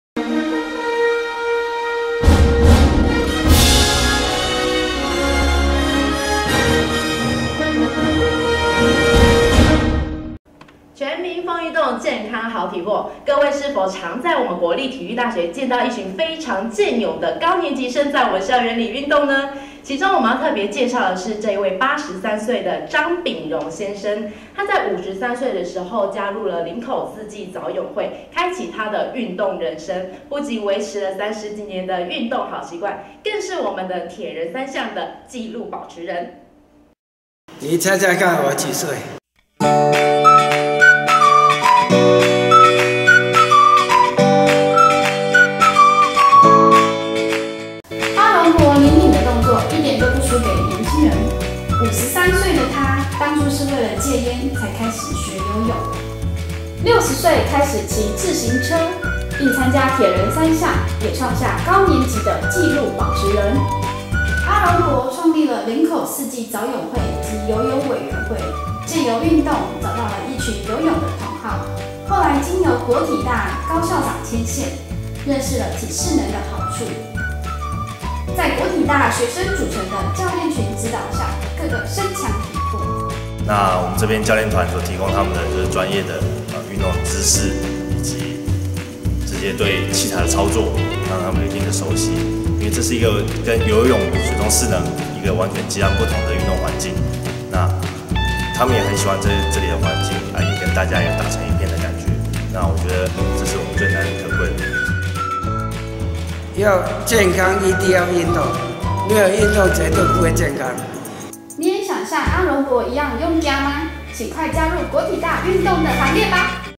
教發新聞「林口四季早泳會」報導
藉由國體大高校長牽線，早泳會的成員結合本校體適能中心，經由專業教練團指導，讓早泳會的大家更愛上運動!